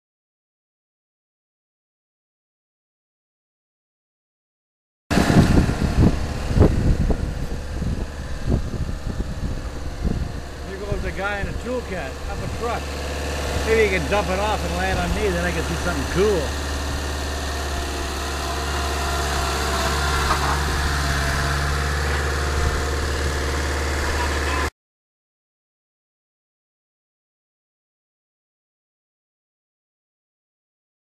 How to load Bobcat ToolCat on Trailer
Careful driving up the ramp to get the BobCat on the trailer.

Float flying Kitfox and Beaver

Great sites

Kitfox files Aircraft info
Free Hoter water info
Weather for pilots
Ontario Ultralights RAA and UPAC